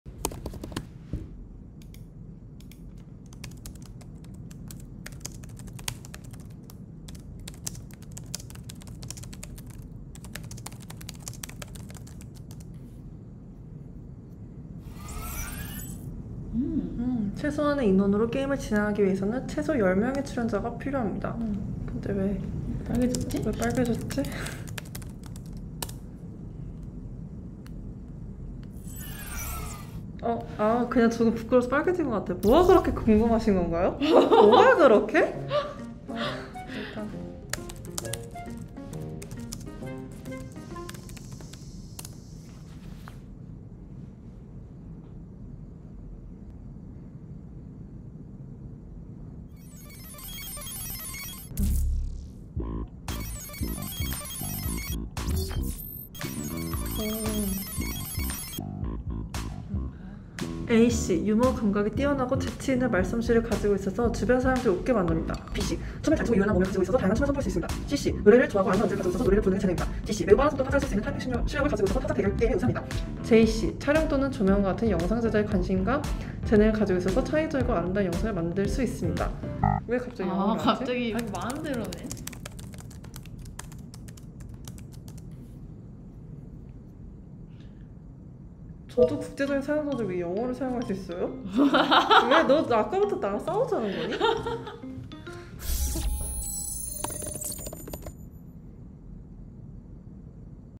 스타 탐색 대작전: AI PD가 선정한 완벽한 출연진 조건
제 프로그램에 어울릴 최고의 스타를 찾기 위해 AI PD 엠파고가 고심하고 있습니다. 어떤 스타가 선택될지 기대해주세요. 우리의 무대를 빛낼 예능 실력과 독특한 개성을 갖춘 인재를 찾아나섭니다.